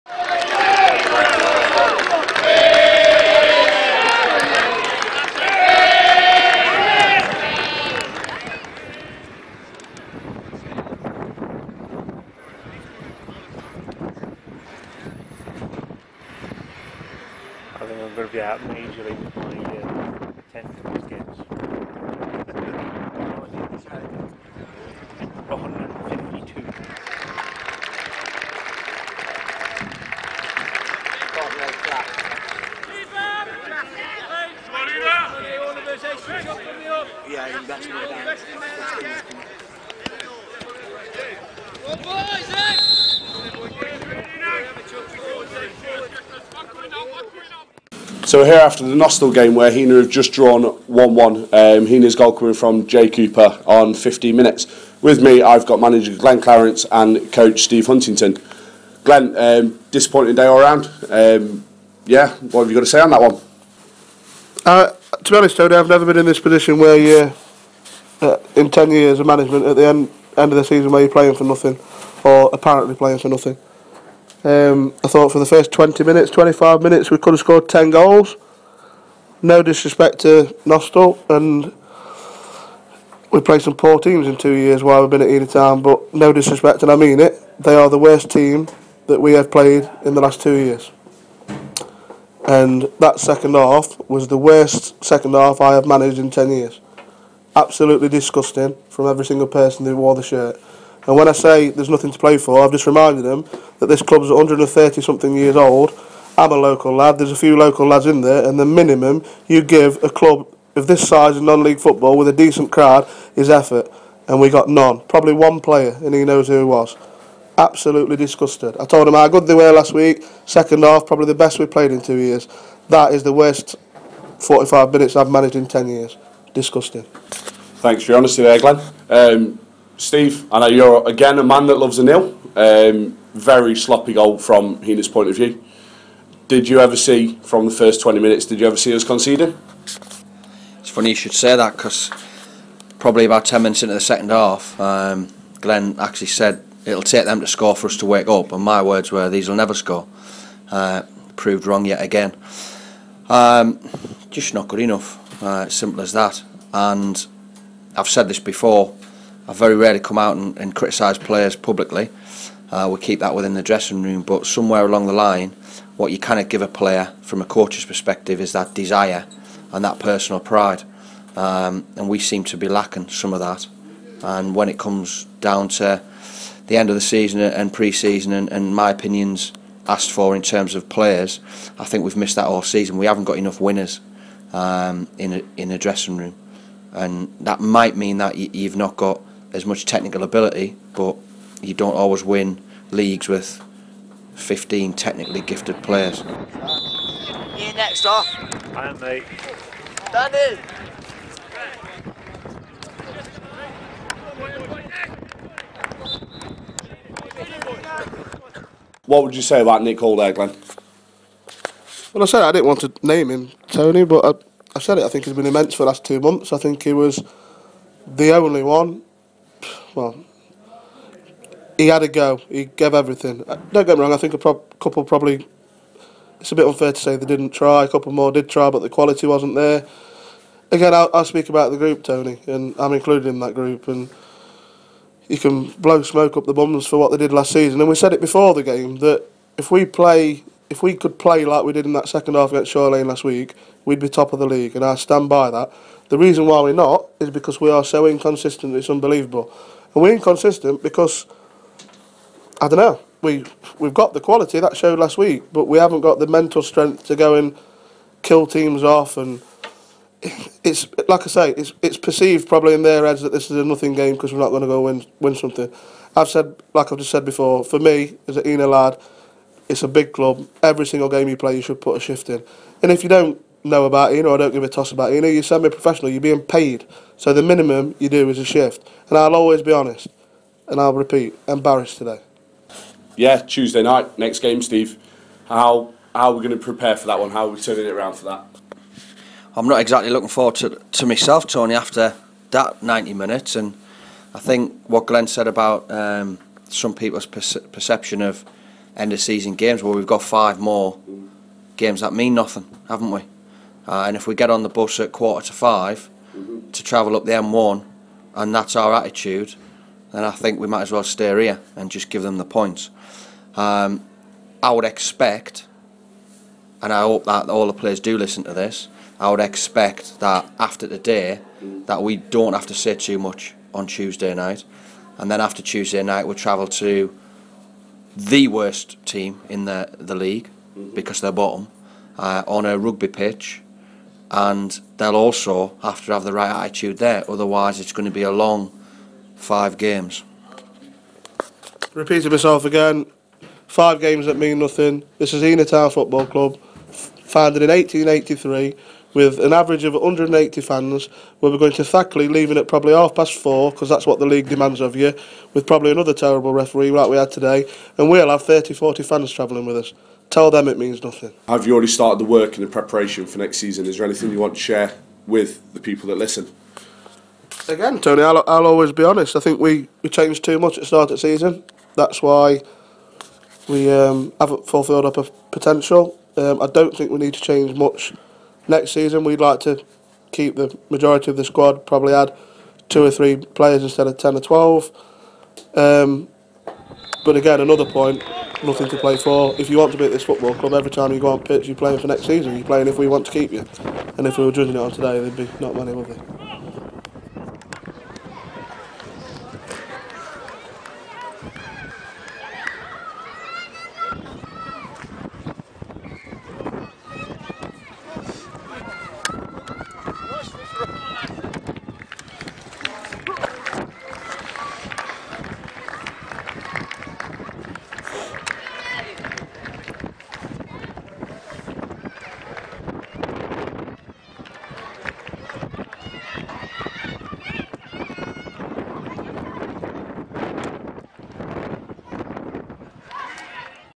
The Lions vs Nostell Miners Welfare FC 11/04/2015
Highlights of the Northern Counties East League Premier Division clash vs @NostellMWFC

Next game vs @ThackleyAFC on 14/4/2015

Tweet your questions and suggestions to @HeanorTownFC podcast or TGTV

From the players and staff @HeanorTownFC. Thank you for your continued support

Thank you HTFC Media Team